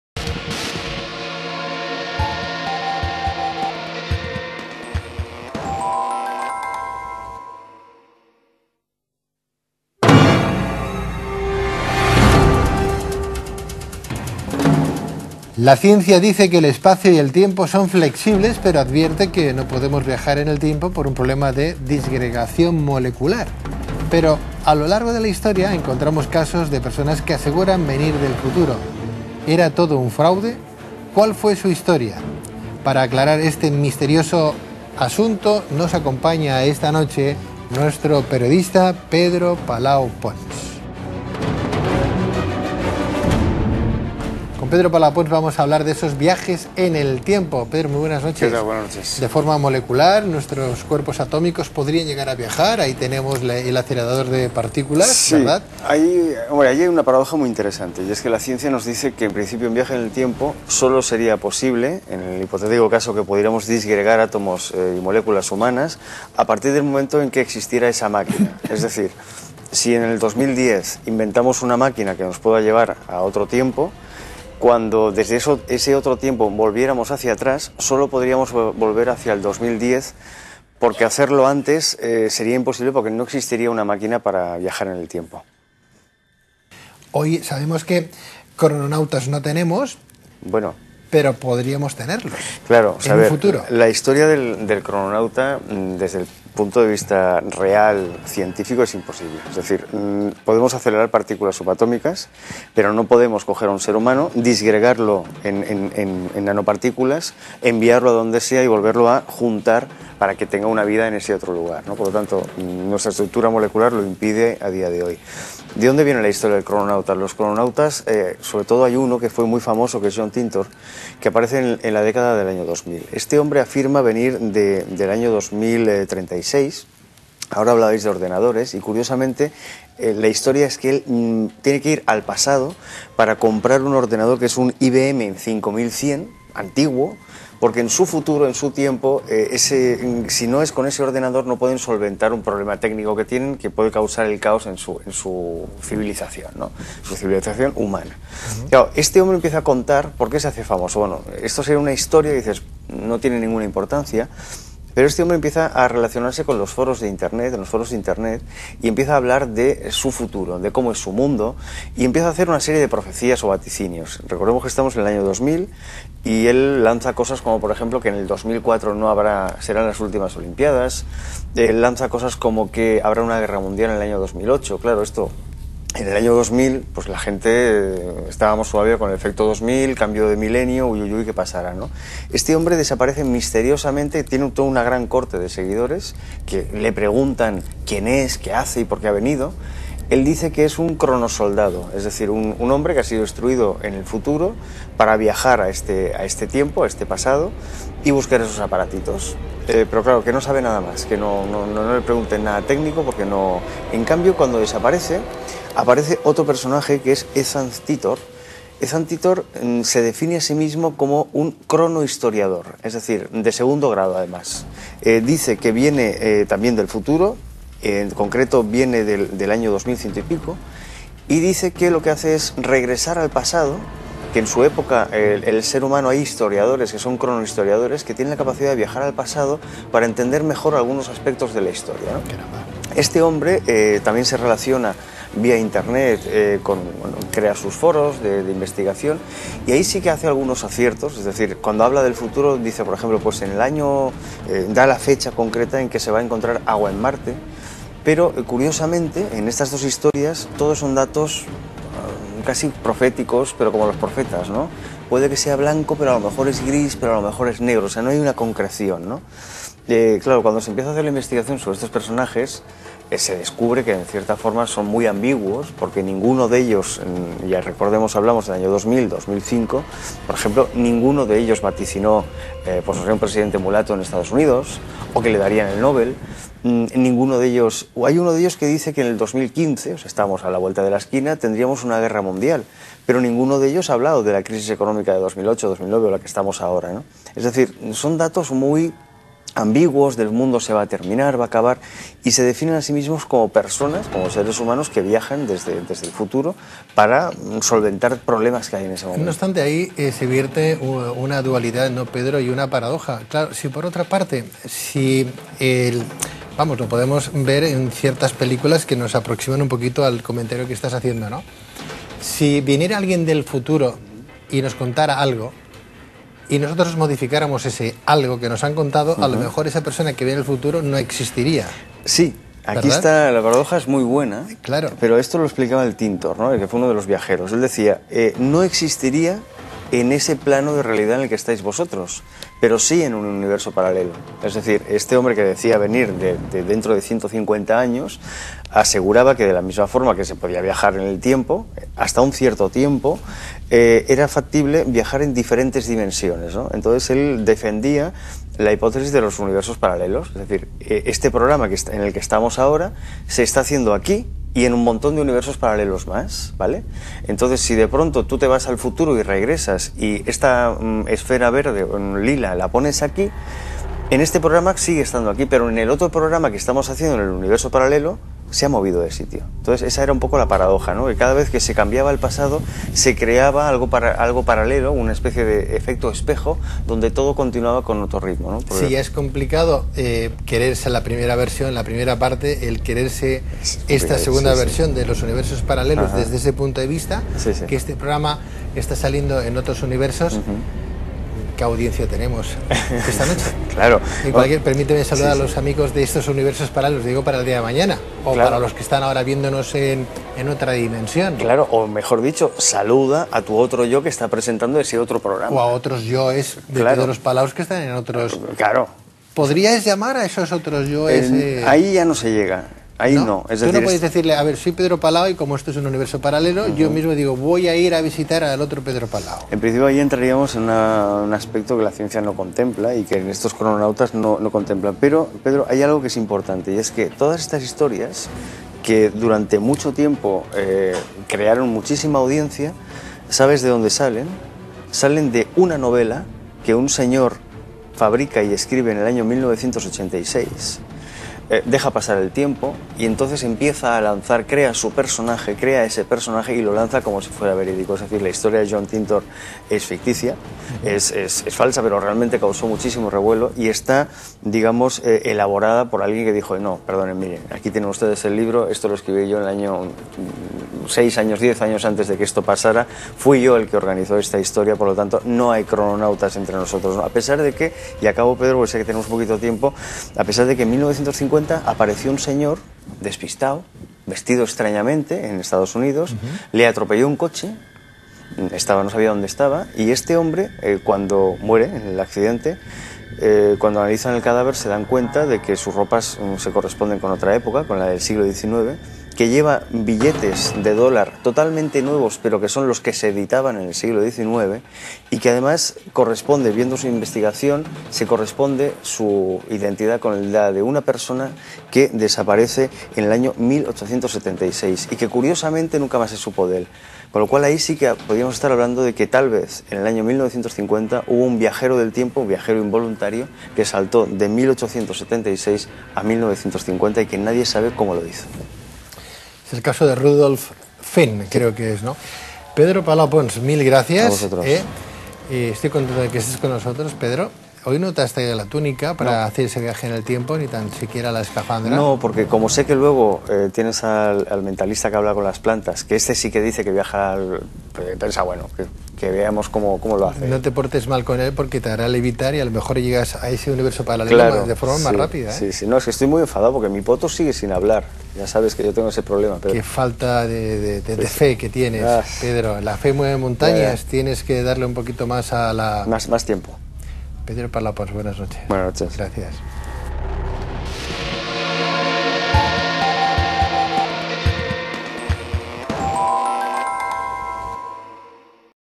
Viajes en el Tiempo
Pedro Palao nos habla de la posibilidad de los viajes en el espacio-tiempo y sus referentes en la Historia.